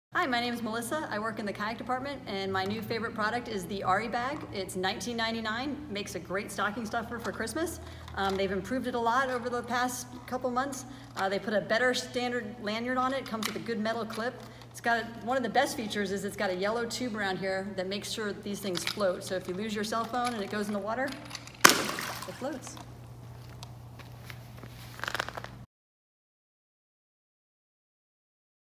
Our Favorite Things: Melissa
Our Favorite Things: Melissa in the Kayak/SUP department discusses her favorite gear item in Bill Jackson's: a waterproof AriBag to protect her smartphone, plus it floats.